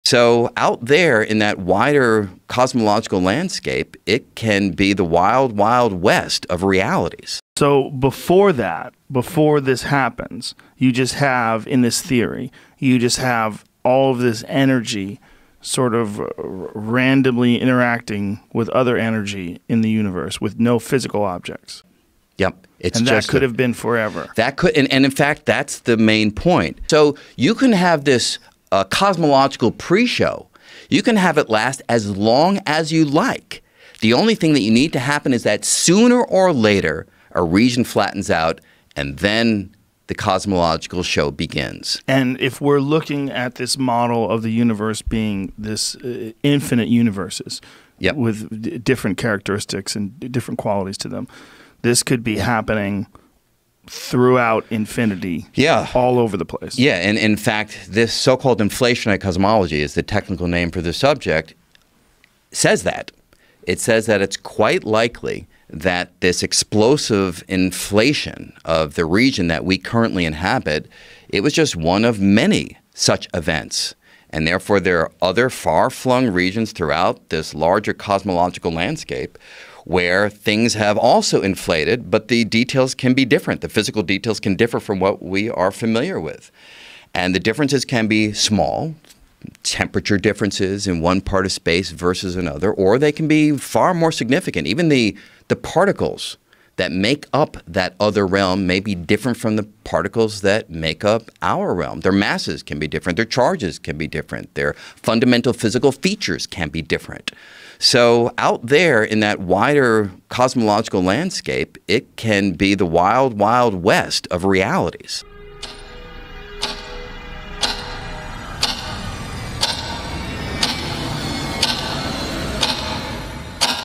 Cosmological Landscape: Exploring the Wild West of Realities
The pre-show to the universe may have lasted for infinity, a landscape of infinite universes with different characteristics, physical features, and qualities. It could be the wild, wild west of realities. What makes up another realm?